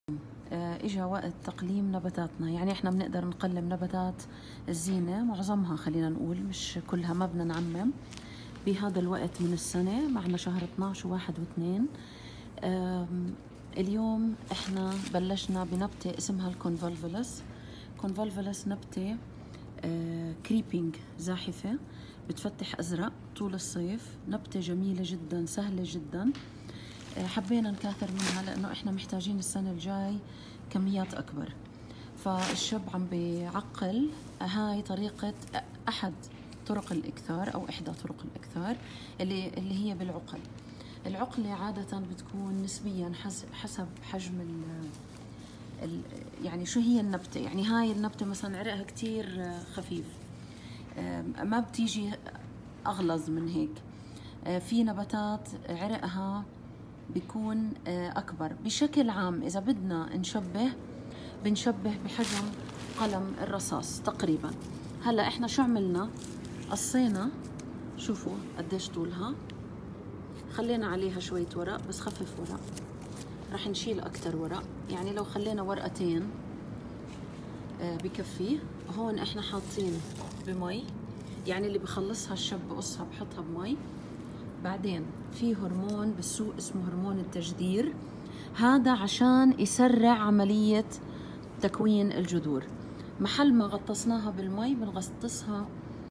تقليم النباتات مع المهندسة الزراعية والكاتبة في مجلة نكهات عائلية أمل القيمري
نصيحة يومية من المهندسة الزراعية أمل القيمري